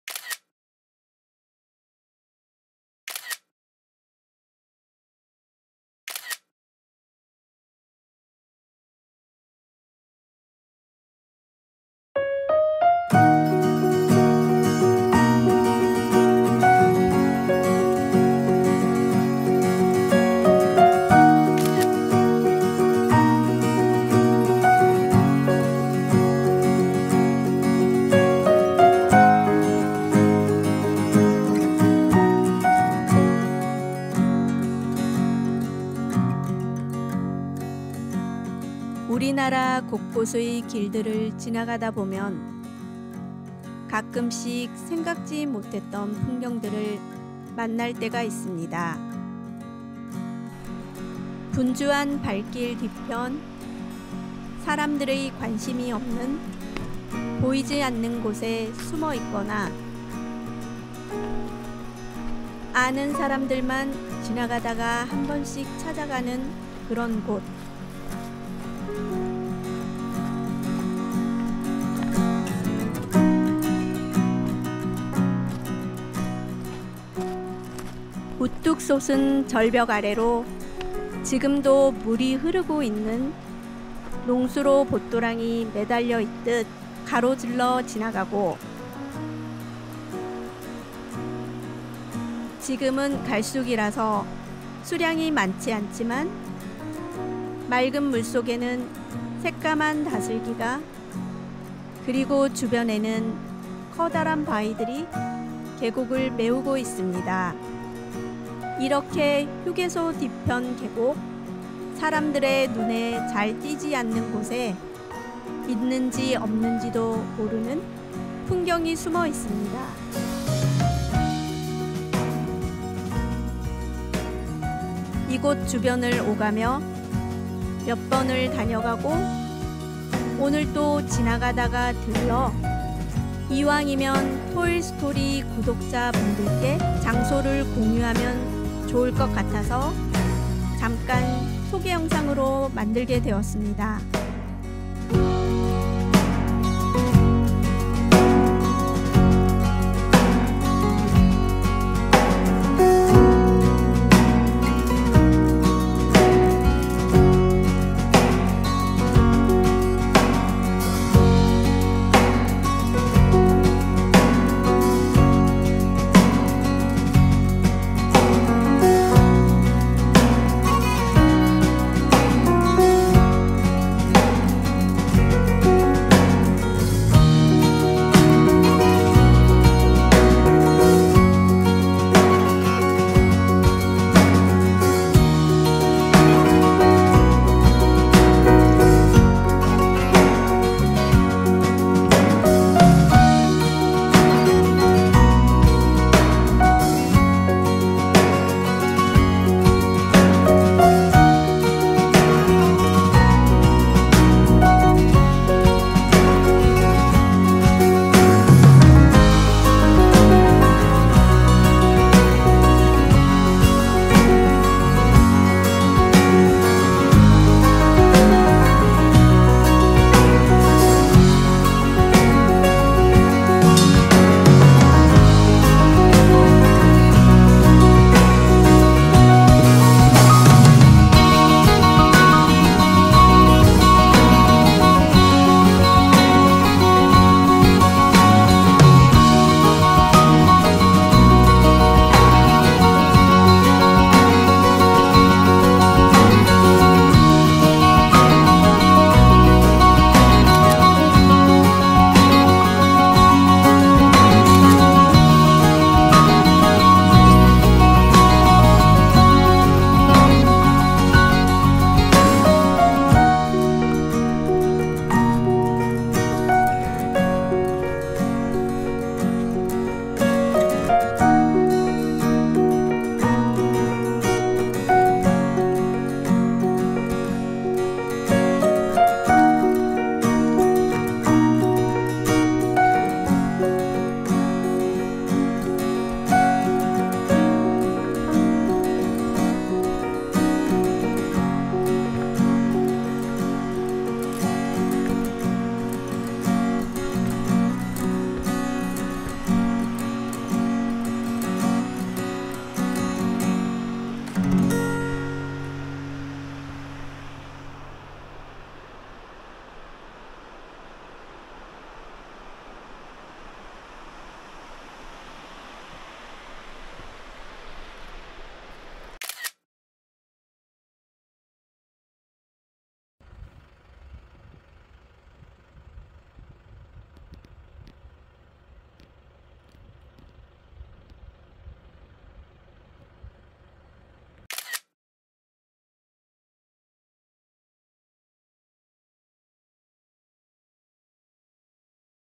[133] 걸어서 5분, 지나가다가 들러 보세요.
5분만 걸어가면 만나는 숨어있는 국도변 계곡풍경